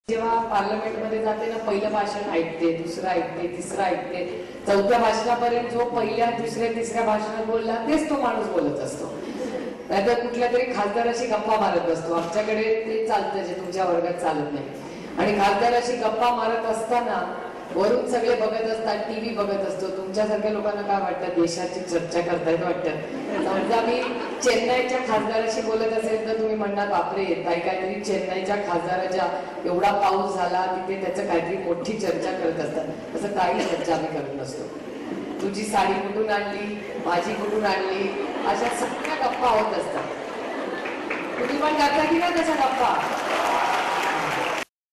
We talk saris in Parliament: Supriya Sule's joke may crash
In what could become a controversial statement, Lok Sabha MP and senior NCP leader Supriya Sule has revealed that parliamentarians do indulge in small talk and gossip even when serious issues are discussed, especially when speeches at Parliament get repetitive.